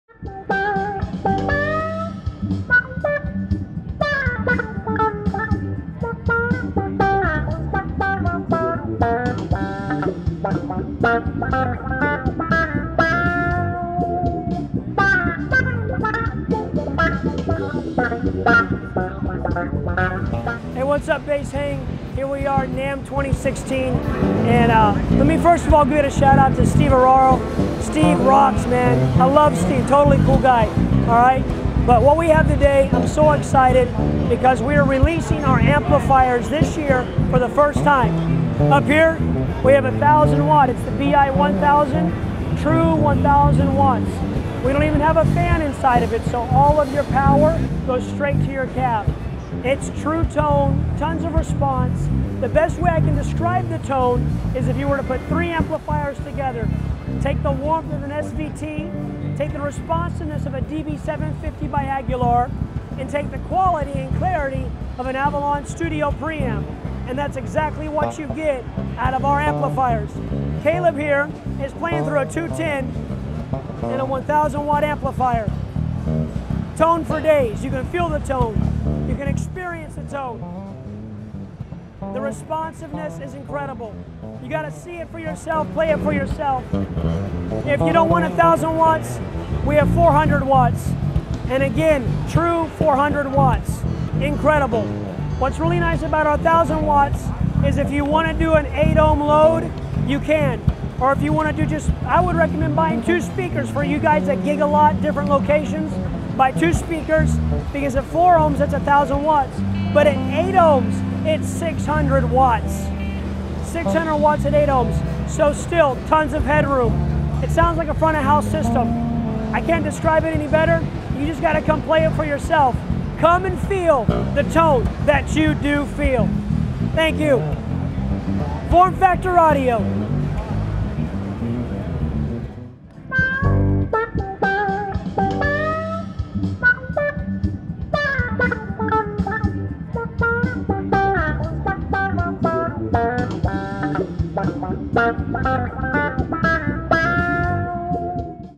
NAMM 2016 Form Factor Audio
Form Factor Audio is stepping up their game with not only offering some killer bass cabinets, but now they're making a powerhouse of a Bass Amp!! We caught up with Form Factors Dane Butler for a quick rundown on their new bass gear.